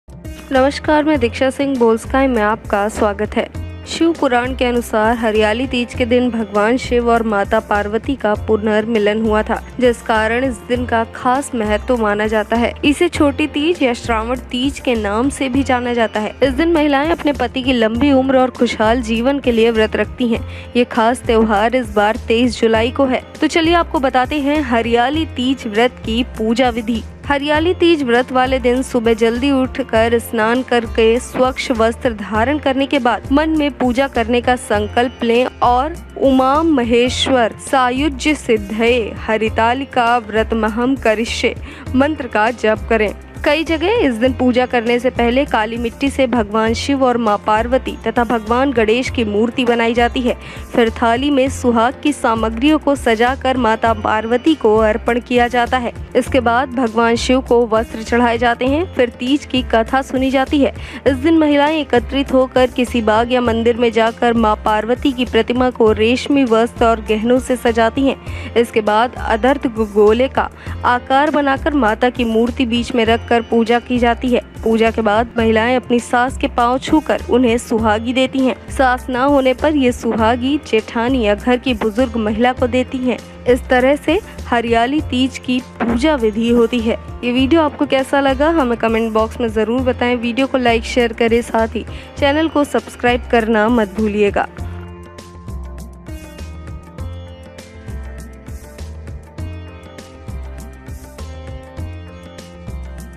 Hariyali Teej 2020: हरियाली तीज का व्रत पहली बार रख रही हैं तो जानें पूजा विधि | Boldsky
Hariyali Teej is fasting for Lord Shiva and Parvati Ji. This festival is celebrated with great pomp in the northern regions of India. Apart from married women, this fast is also observed by girls.

हरियाली तीज पर भगवान शिव और पार्वती जी के लिए व्रत किया जाता है। भारत के उत्तरी इलाकों में इस पर्व को बड़े ही धूमधाम से मनाया जाता है। ये व्रत शादीशुदा महिलाओं के अलावा कुंवारी कन्याओं के द्वारा भी रखा जाता है।